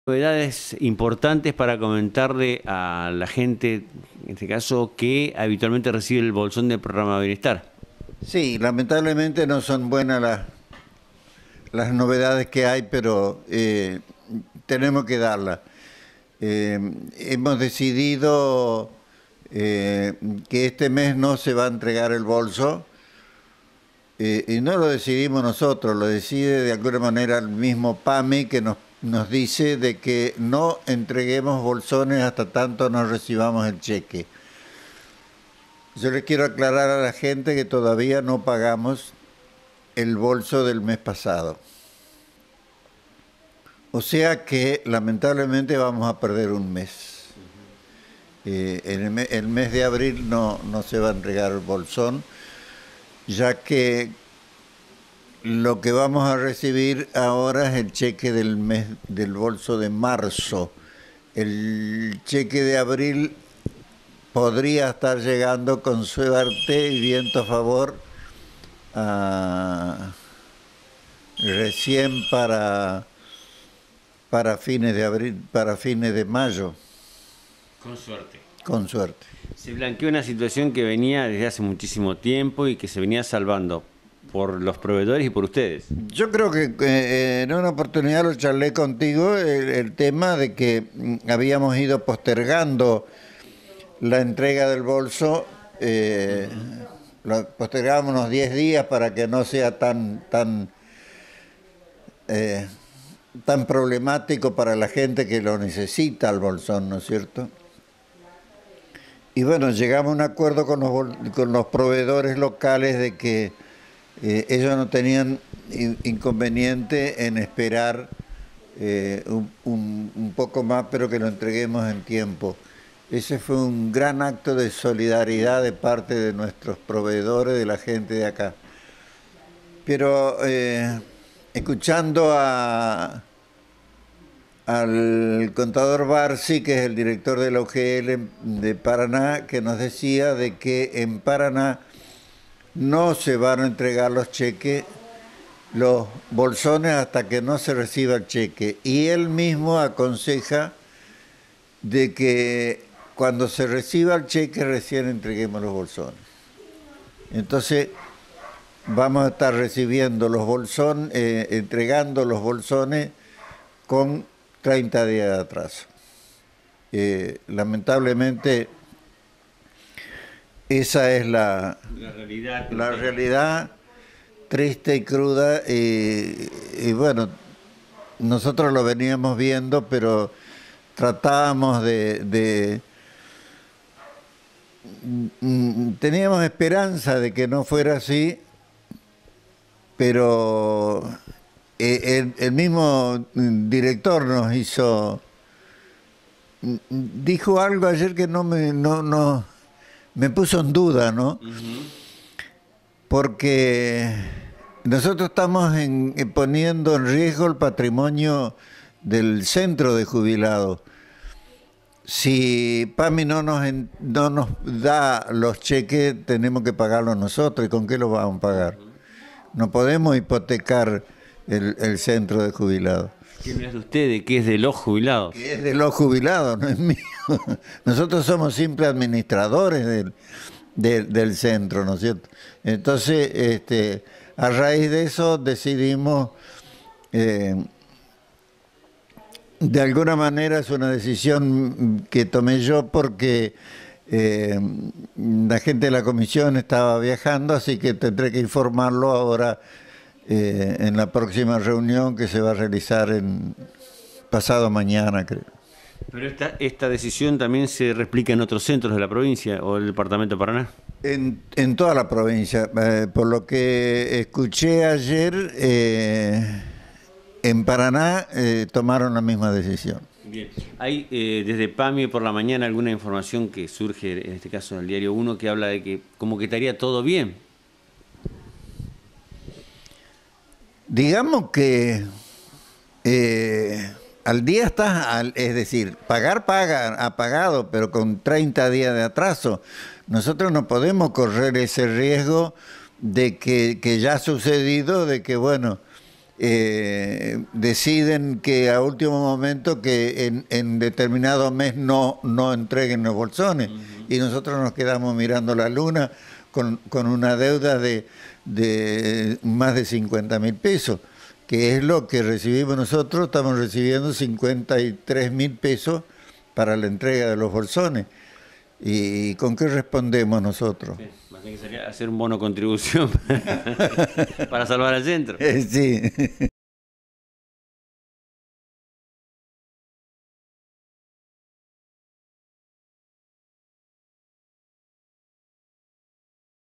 Jorge Saín, Presidente del Centro de Jubilados Nacionales de Cerrito
Manifiesta su preocupación ya que este mes no se podrá entregar el Bolsón Probienestar de PAMI a los jubilados, debido a que el dinero no llega a tiempo.